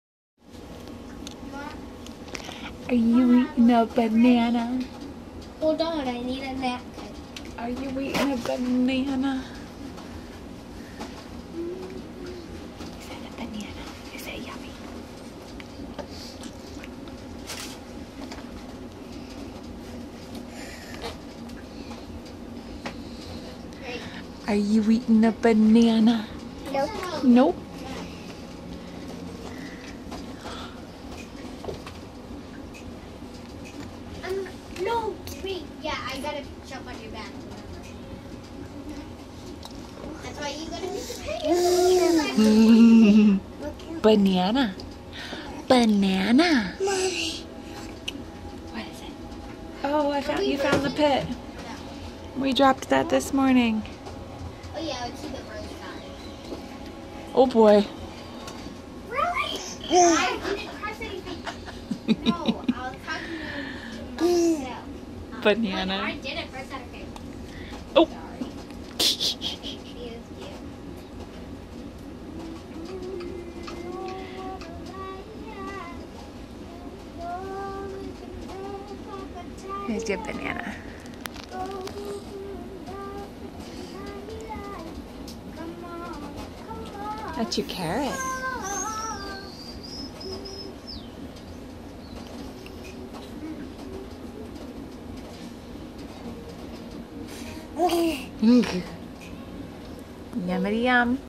Violets First Banana
Almost 10 months old and still hadn't tried a banana! Unheard of!